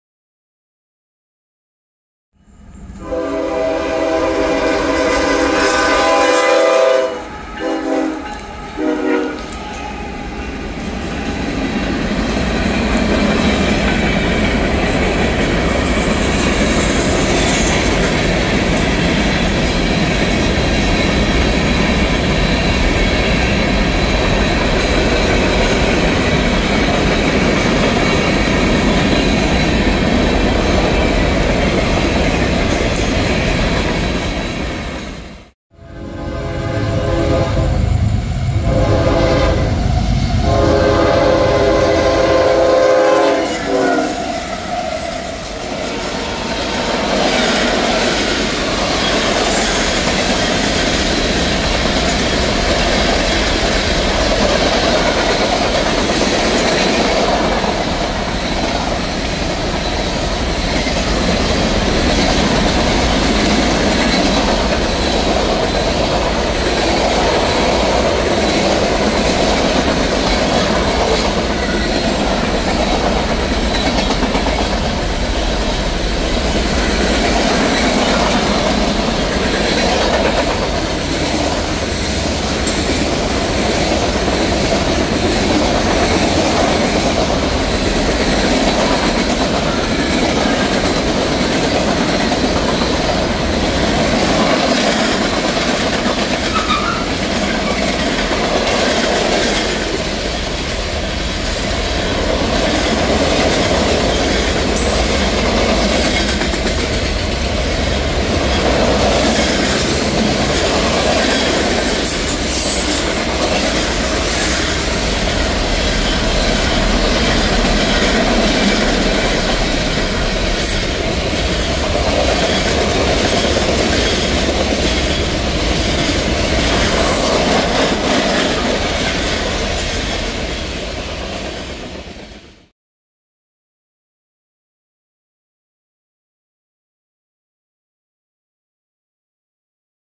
CSX Manifest Train Speeding Through South Carolina!
Caught the end of CSX M693 departing Augusta and noticed a EMD SD70MAC.we decided to head to Allendale Sc then on to Estill Sc where CSX M693 was ina hurry to get to Waycross Ga

CSX 886 Lead
CSX 136
CSX 4547 SD70MAC

Augusta 4, 2024

0:00 Intro
0:04 Allendale Sc
0:35 Estill Sc